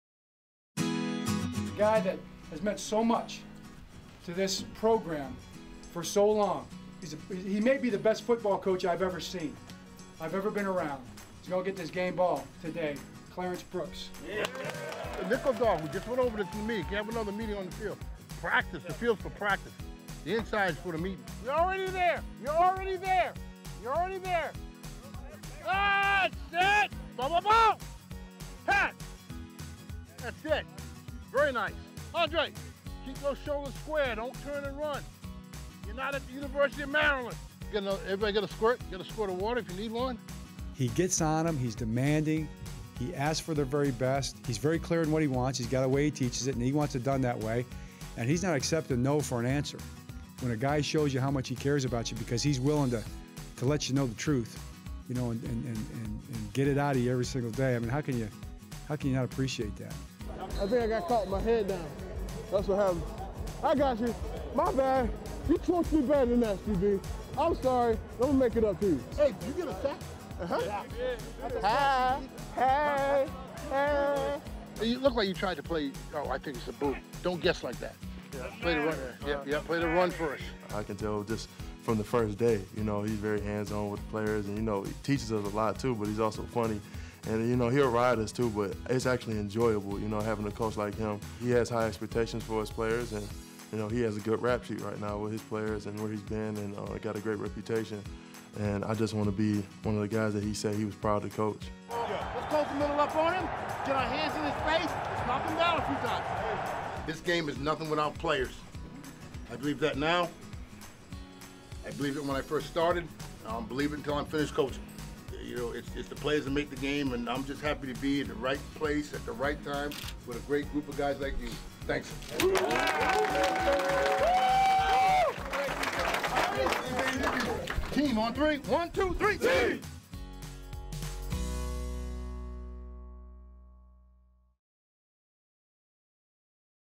Best Of Clarence Brooks | Wired | Baltimore Ravens
Take a look back on the best 'Wired' moments from the Ravens' longest-tenured assistant coach.